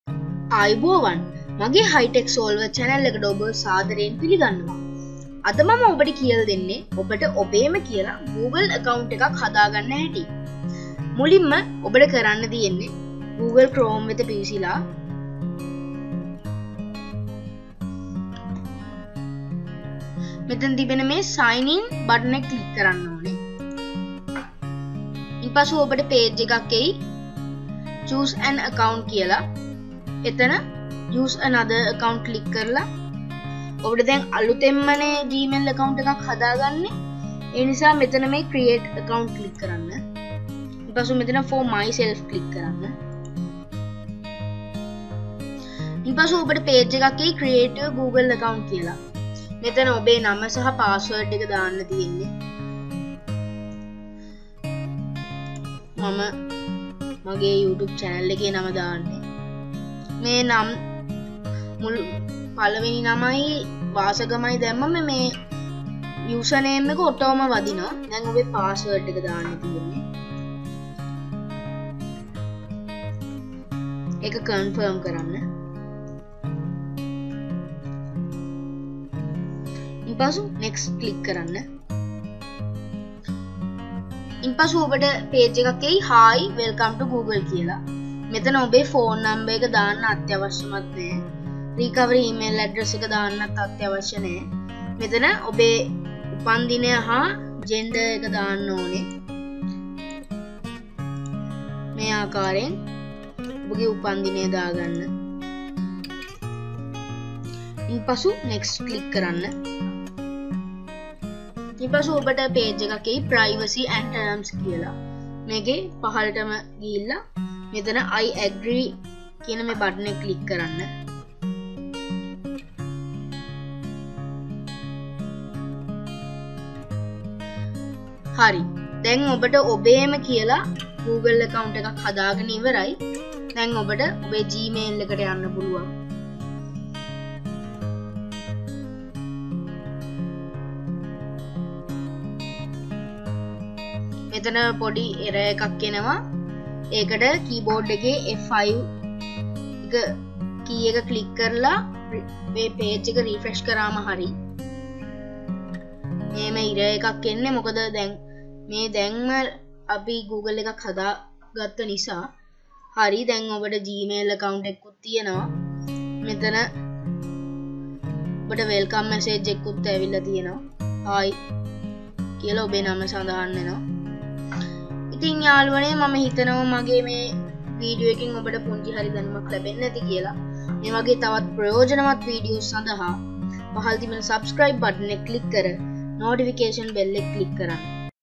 How to make a new google account for beginners. (Hightech solver)
This video will explain to you how to create a google account for beginners step by step in 3 minutes.